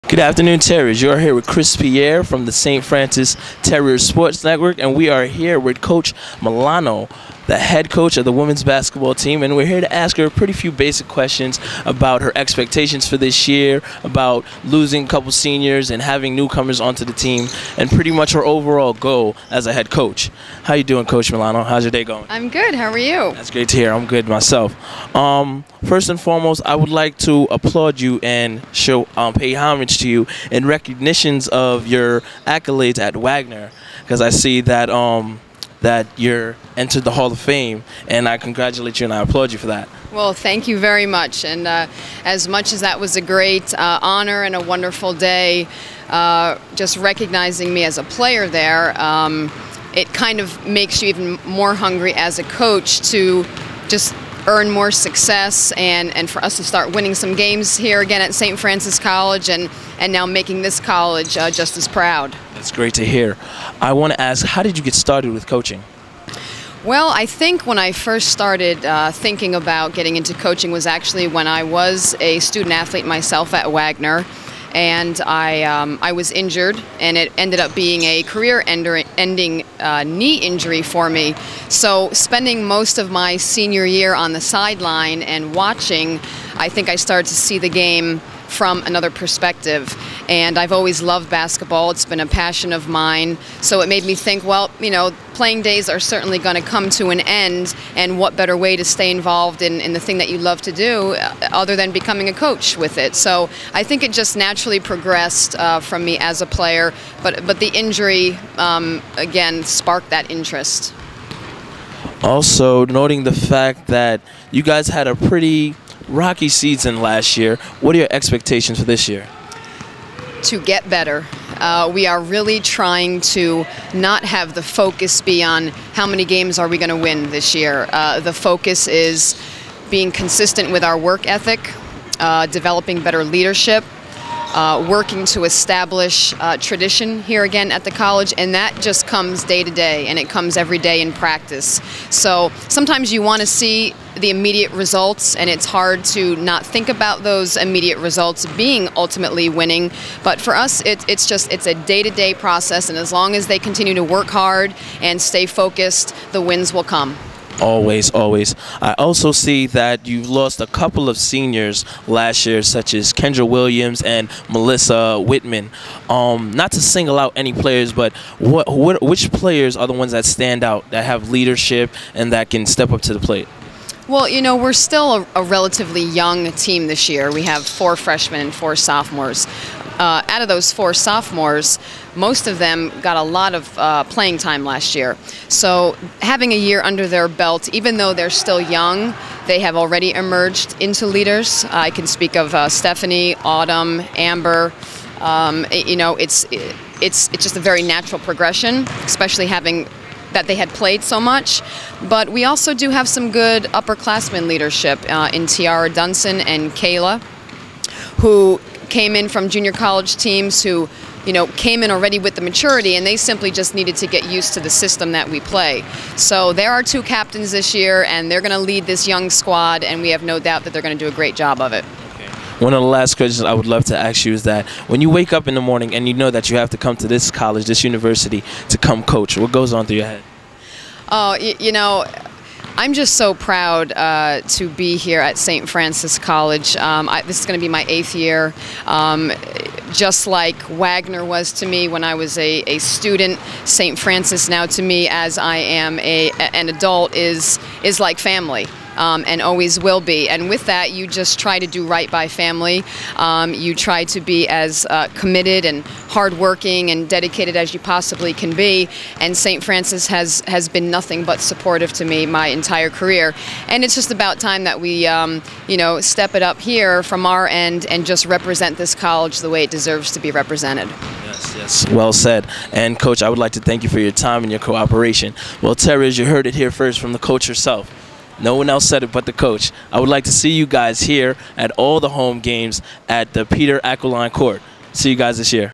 Coach Brenda Milano Previews 2010-11 Season
Coach Brenda Milano talks with Chris Pierre of the Terrier Sports Network about the 2010-11 St. Francis College Women's Basketball Team.